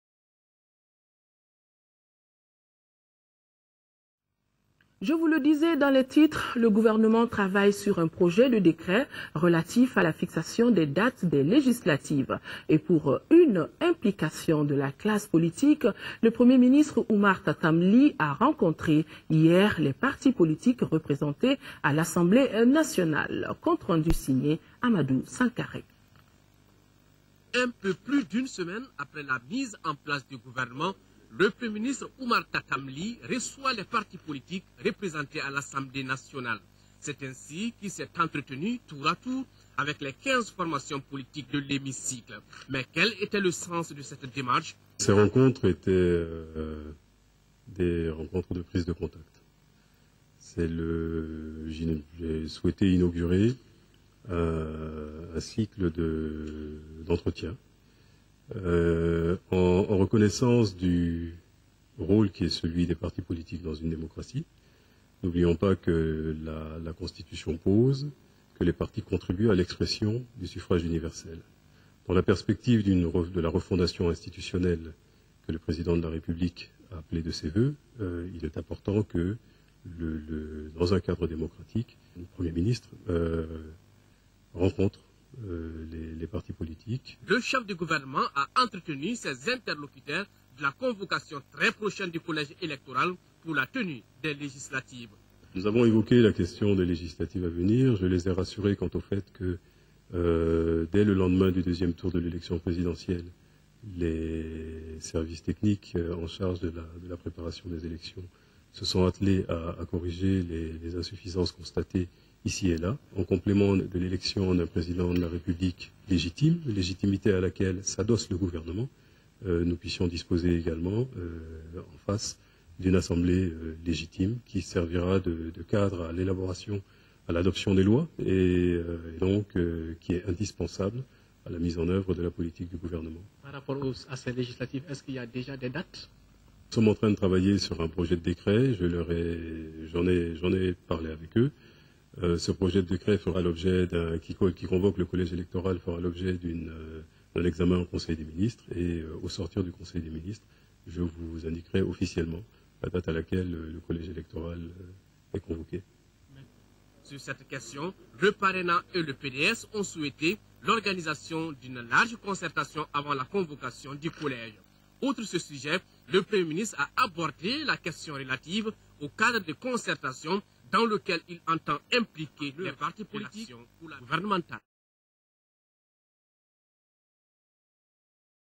En vue du projet de la date des législatifs le P M Oumar T rencontre les partis représentes à
En vue  du projet de la date des législatifs le P M Oumar T rencontre les partis représentes à l'Assemblée Nationale